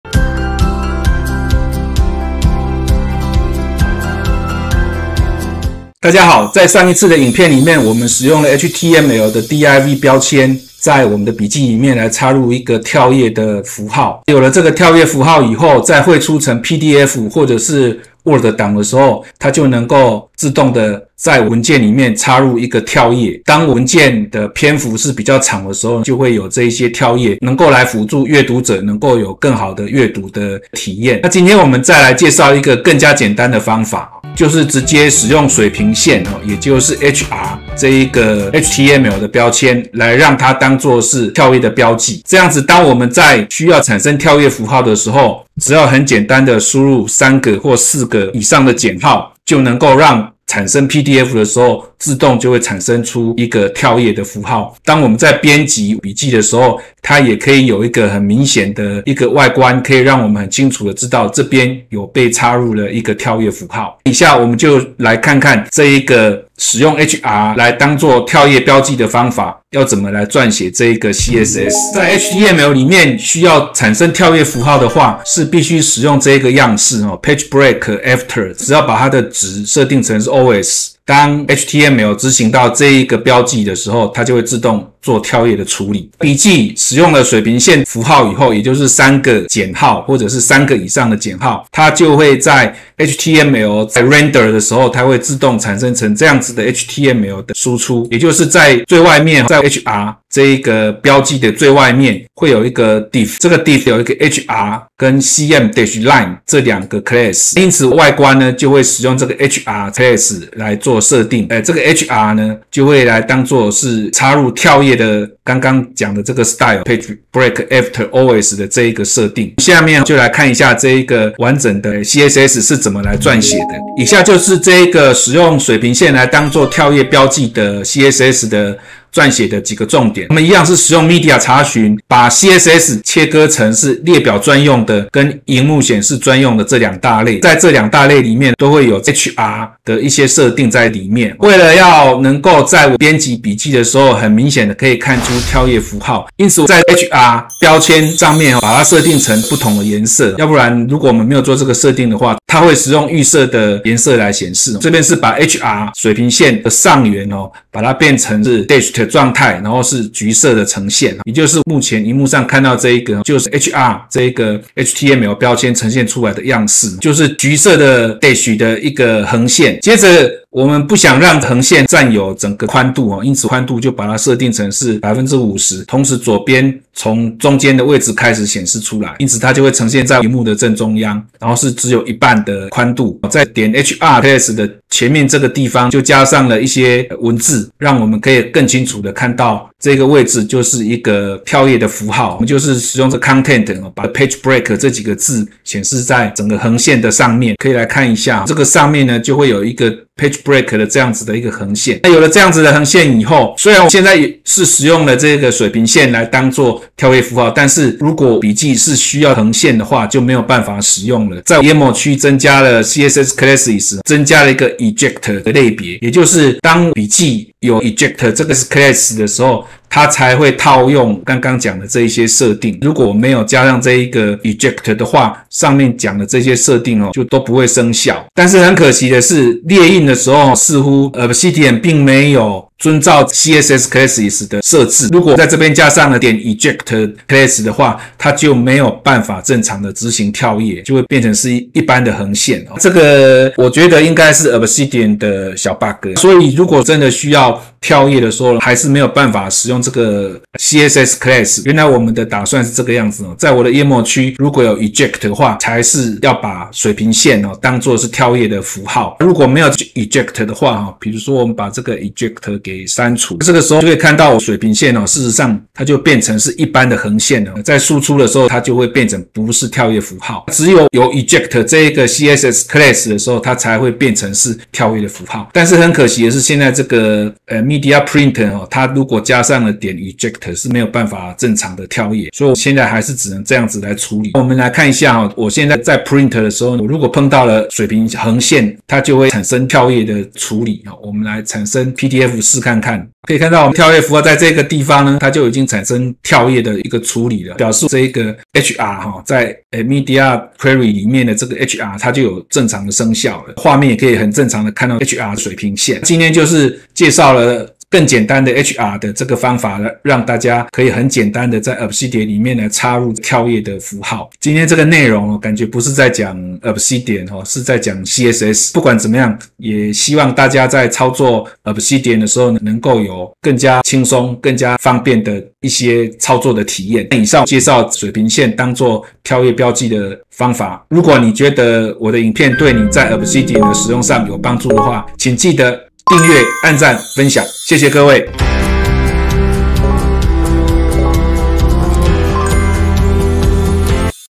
Obs099 | Obsidian跳頁分隔線的簡單作法：使用HR標籤（水平線）
在上一次的介紹裡使用HTML的div標籤在筆記裡插入跳頁標記，在匯出成PDF檔案時能正常的切割內容。這次介紹另一個更簡單的方法：使用水平線（HR，Horizontal Ruler）當做跳頁標記，在筆記裡只要輸入3個以上(含)減號就代表 🦘此處跳頁 了。

💡 解說文章:  jdev.tw/blog/7714/

⚠️ 注意！
3個以上(含)減號上方必須有一個空行，以免被解析成二級標題H2了。